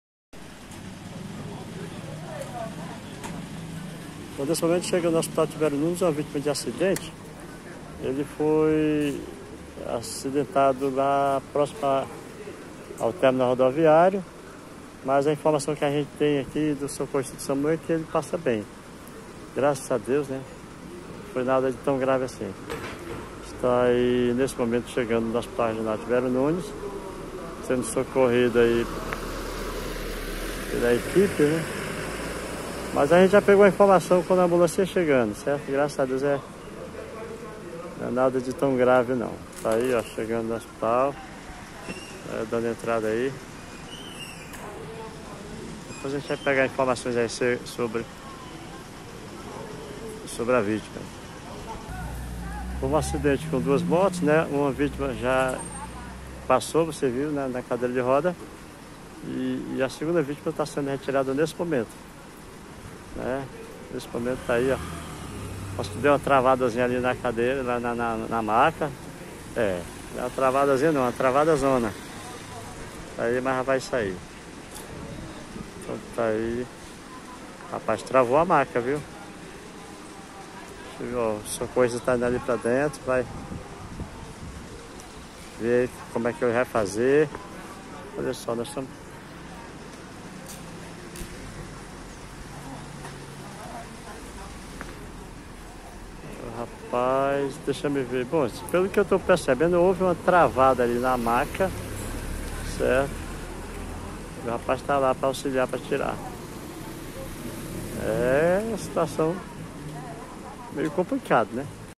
Acidente entre duas motos deixa seus condutores feridos
No início da tarde de hoje, em frente ao Terminal Rodoviário de Floriano, um acidente envolvendo duas motocicletas deixou os dois condutores dos veículos feridos.
Eles foram socorridos por uma equipe do Samu, conduzido de para o Hospital Regional Tibério Nunes, onde foram identificados como Denilson Costa Sousa, 20 anos e José Venâncio da Silva, de 56 anos. Denilson deu entrada numa cadeira de rodas, enquanto o Sr Venâncio foi conduzido numa maca com o braço fraturado.